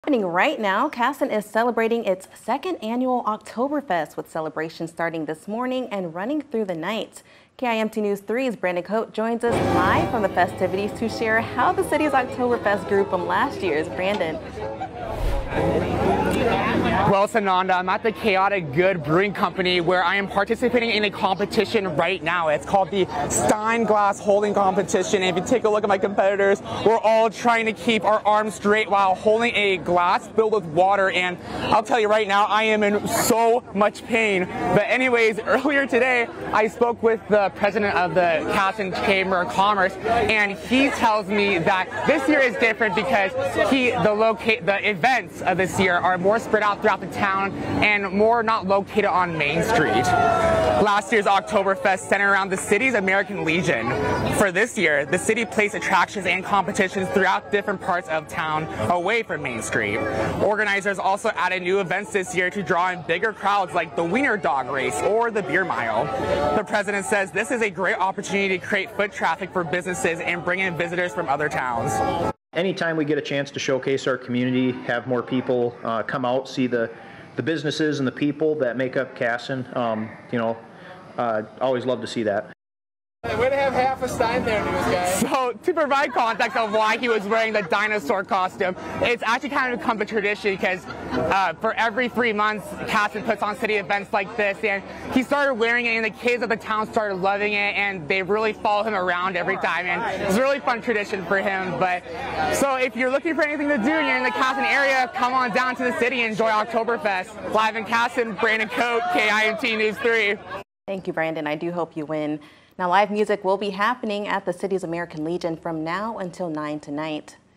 Kasson celebrates 2nd Annual Oktoberfest
Kasson celebrated its 2nd Annual Oktoberfest Saturday and the city says it changed the way they set up its festivities by spreading activities throughout different parts of the town.